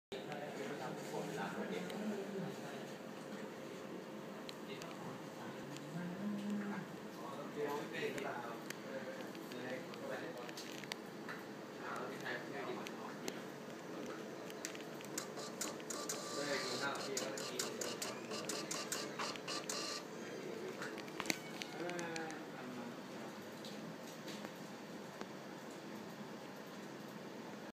MaYom with Arduino
MaYom provide serial interface to allow Microcontroller or Computer work with USB Thermal Printer easier. Support USB Thermal Printer compatible with EPSON ESC/POS.